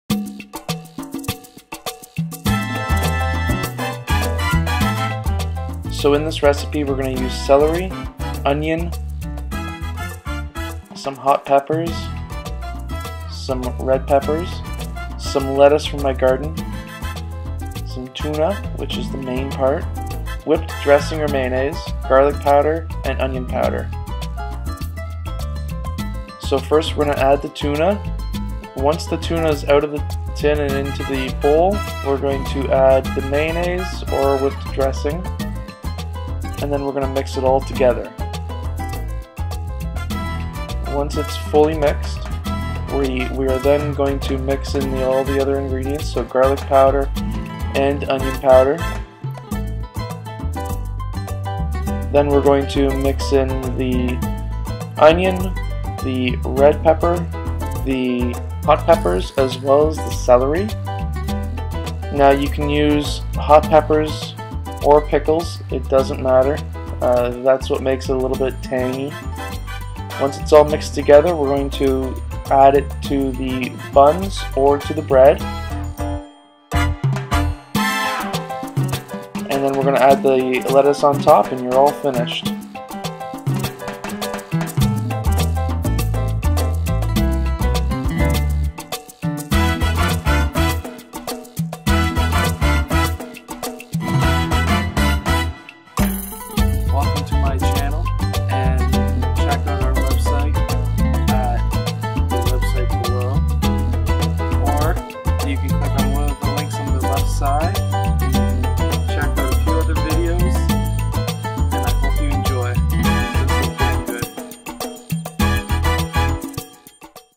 Tangy Tuna Salad Sandwich Recipe
This is a recipe for Tangy Tuna Salad Sandwiches, this is a great recipe that is not only simple but also healthy. This Tuna Salad recipe includes the following ingredients:

1 Tin of Tuna
Handful of:
Onion
Red Pepper
Celery
Hot Peppers and/or Pickles

1 TBSP of Mayonnaise